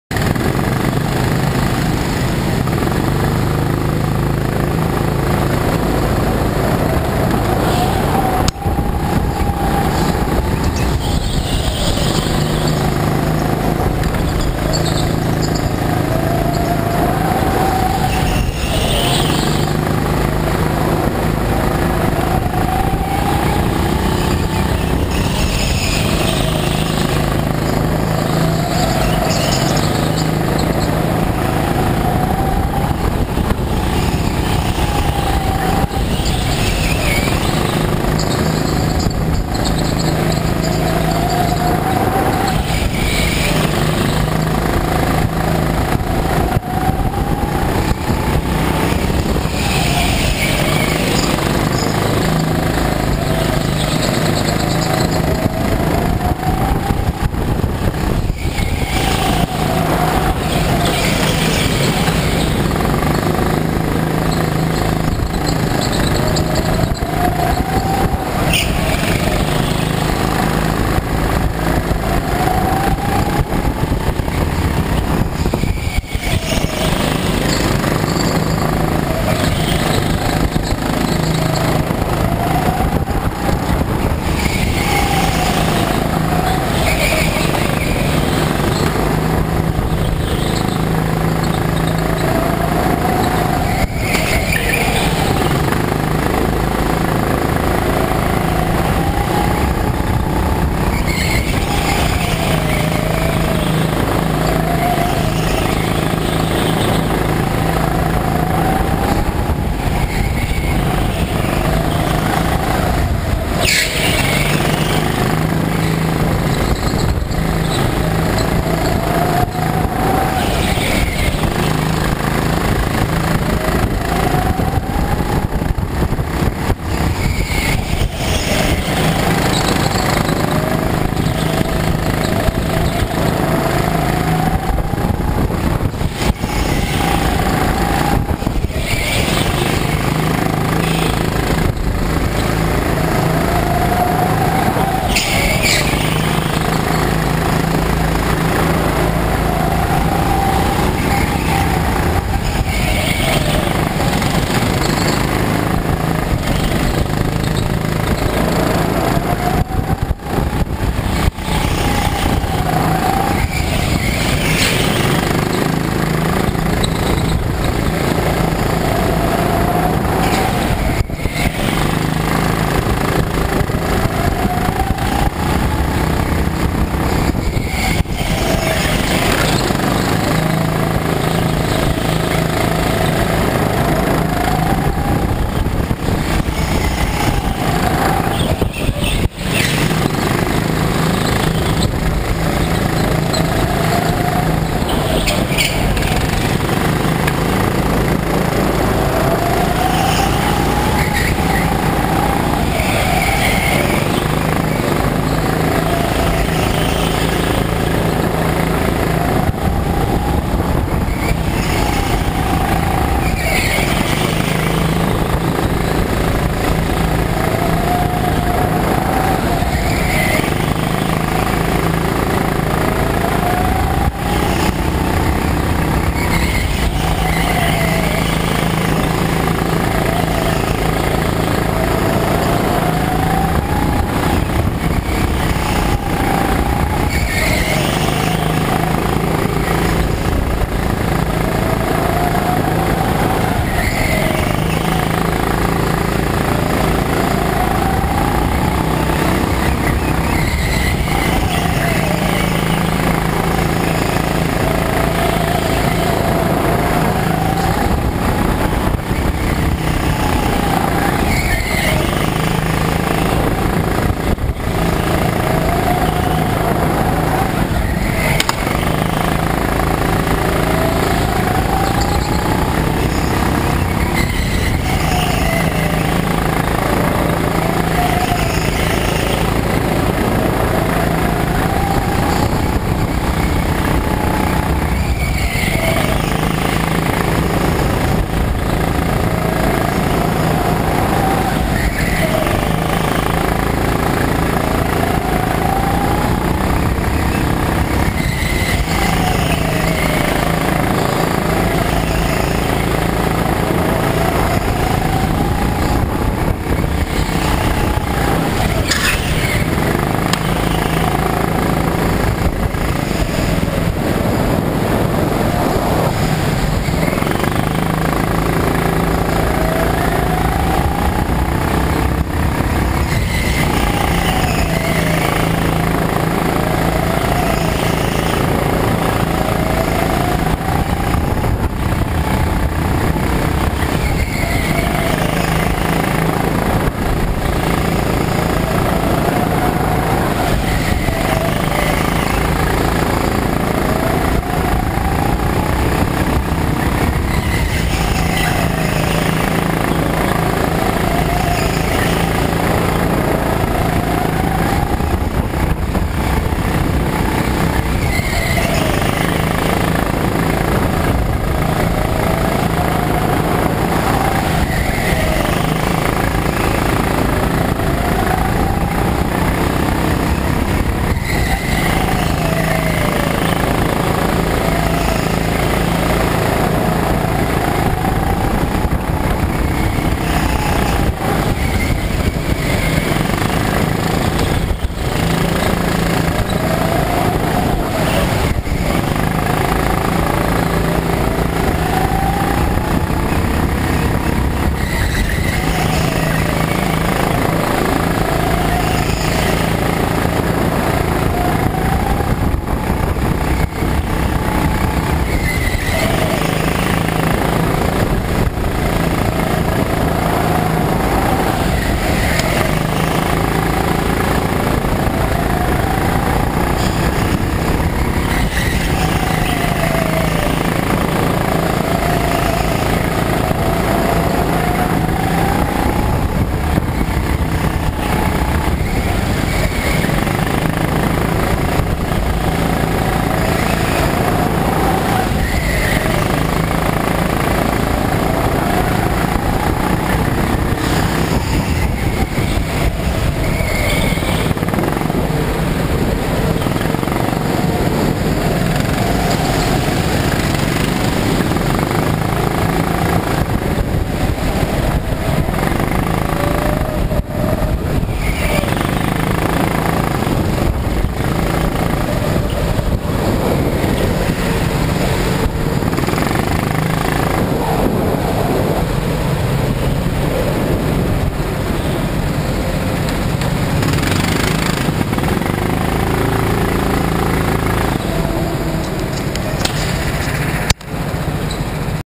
Full Throttle Birthday Race #2
Video from my cousins birthday party at Full Throttle Karting in Cincinnati